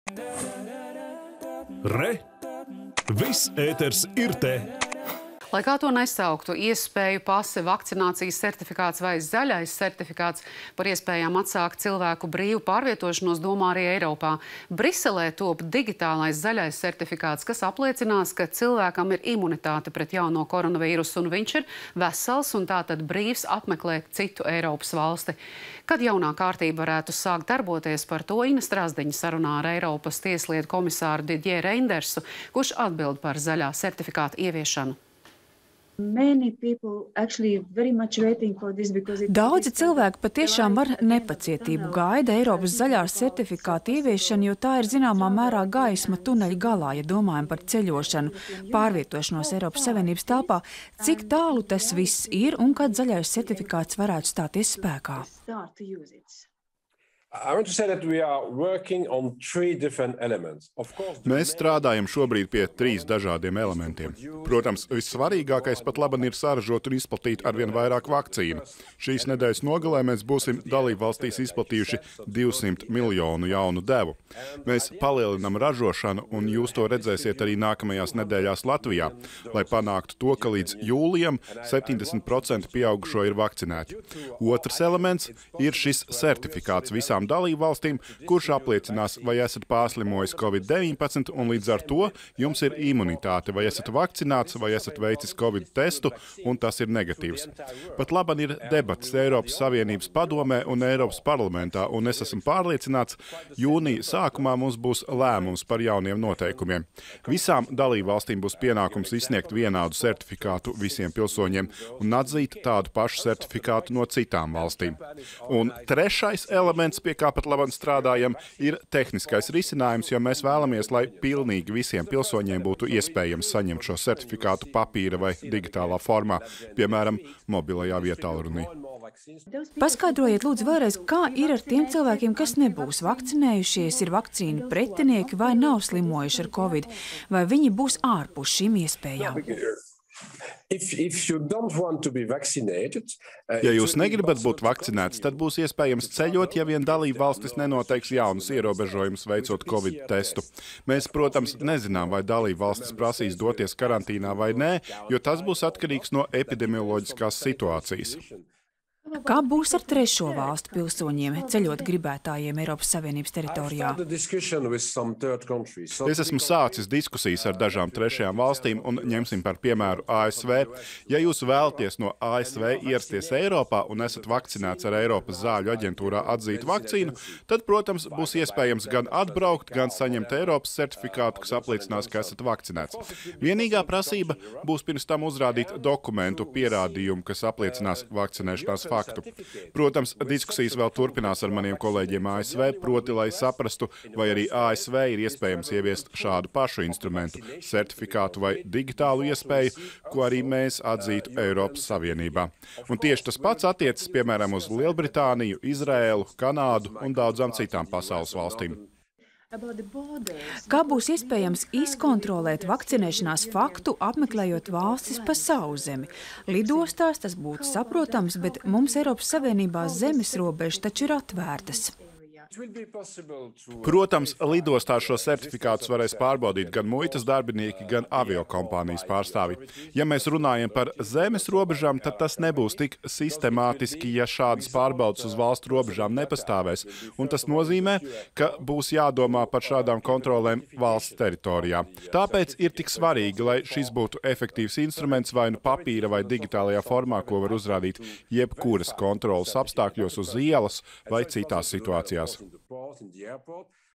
Eiropas komisārs: līdz jūnijam jābūt lēmumam par zaļo sertifikātu
Lai kā to sauktu – iespēju pase, vakcinācijas sertifikāts vai zaļais sertifikāts – par iespējām atsākt cilvēku brīvu pārvietošanos domā arī Eiropā. Briselē top digitālais zaļais sertifikāts, kas apliecinās, ka cilvēkam ir imunitāte pret jauno koronavīrusu, viņš ir vesels un tātad brīvs apmeklēt citu Eiropas valsti. Par to, cik tālu šis process ir un kad viss varētu sākt darboties, kolēģe Ina Strazdiņa sarunājās ar Eiropas tieslietu komisāru Didjē Reindersu, kurš atbild par zaļā sertifikāta ieviešanu.